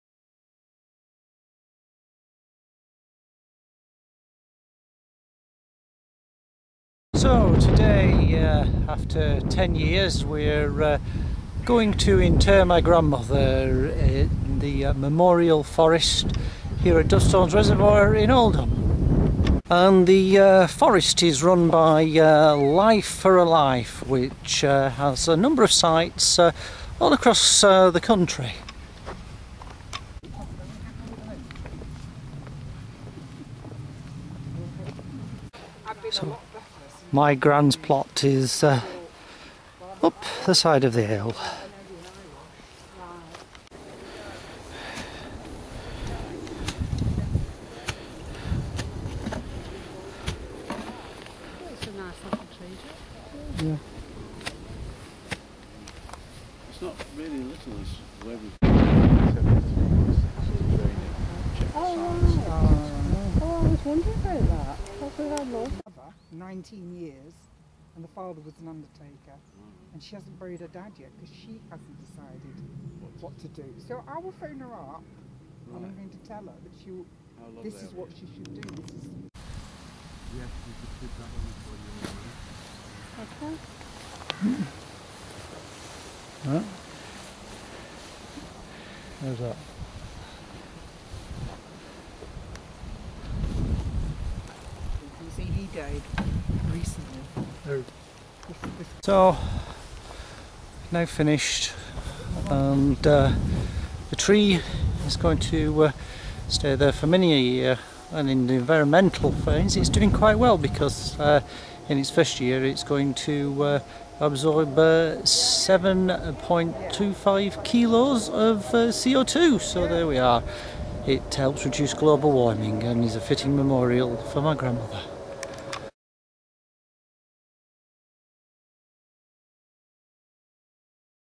2 September 2011 - VB203 - Memorial Forest
Video Blog 203: Today we went to bury my grandmother's ashes under a tree in the memorial forest at Dovestones reservoir in Oldham run by the charity "life for a life" more details can be found